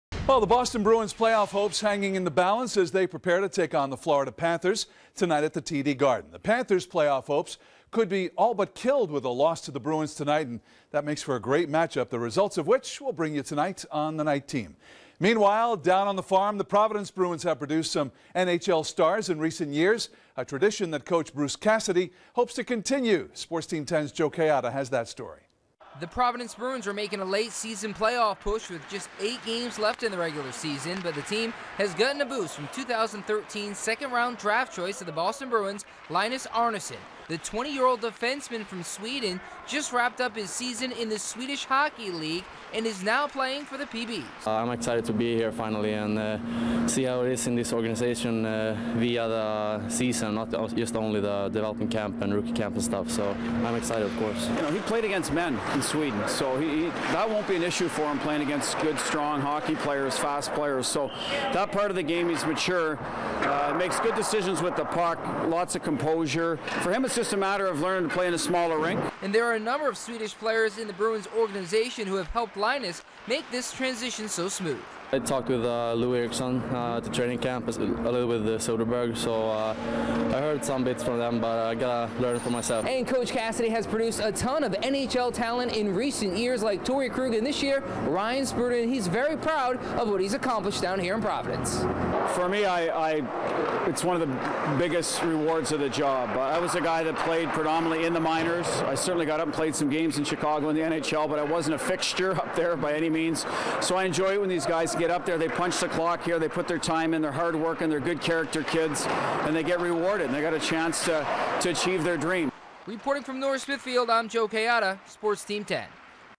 Providence Bruins get a boost from Arnesson
Linus Arnesson, 20, just wrapped up his season in the Swedish Hockey League, and is now playing for the Providence Bruins.&nbsp;